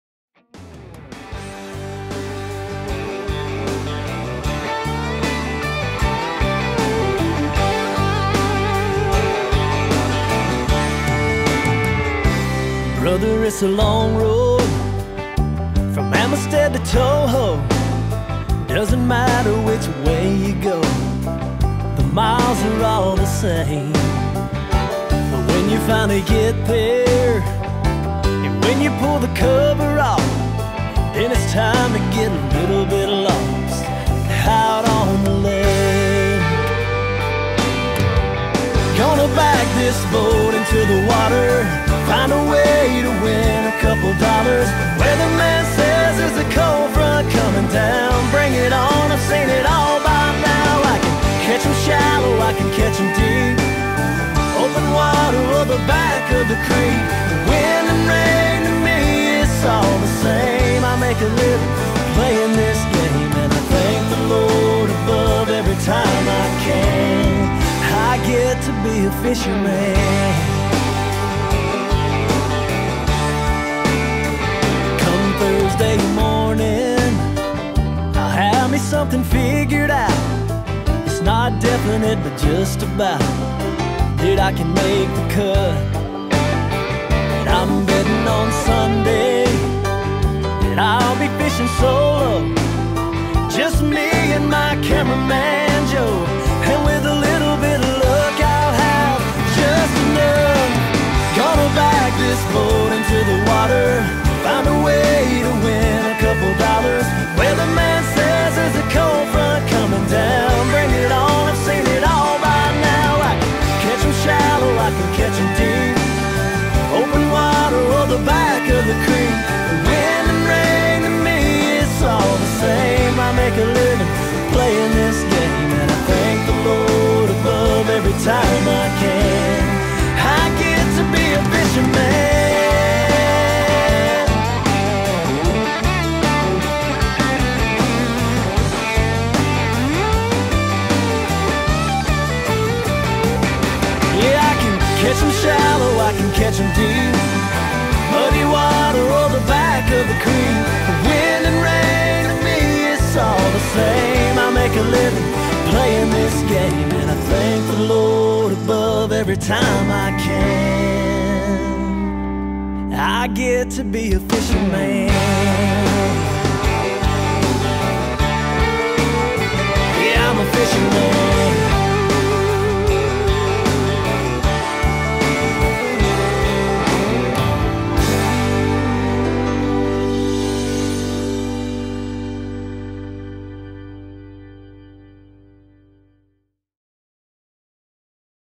Cairns Barra.wmv
Nice little Cairns Barra, some country music and a great time with my Uncle!